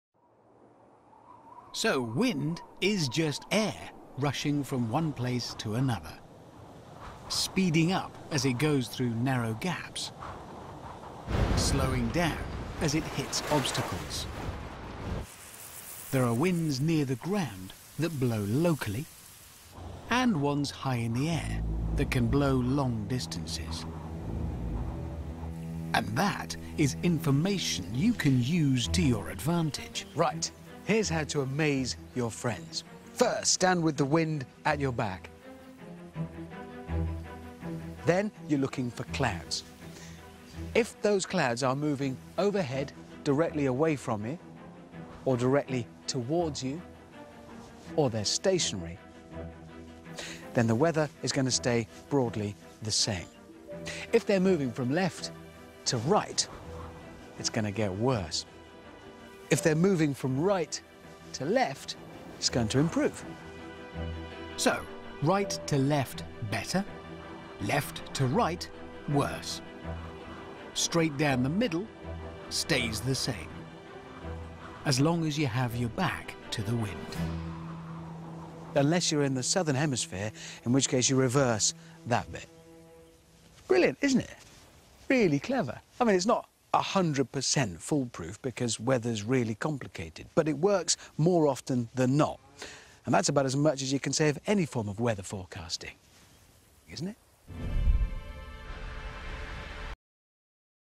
Predicting Weather
Predicting your local weather, by just using the wind.
Remember If you live in the southern hemisphere, reverse the left to right predictions.